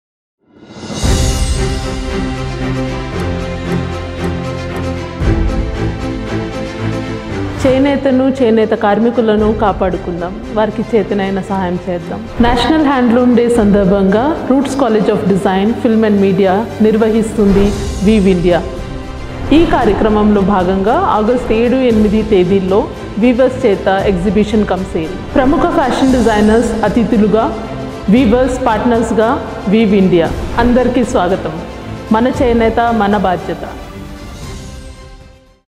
Weave India - An initiation by the Roots College of Design, Film, and Media
Roots Collegium introducing 'Weave India', initiated by the Roots College of Design, Film, and Media. This event will promote and bring the spotlight back to the traditional art of weaving, and promote weaving as an art and weavers as artists.

✅ Do join us for this event!
📅 Date: 7th and 8th August, 2021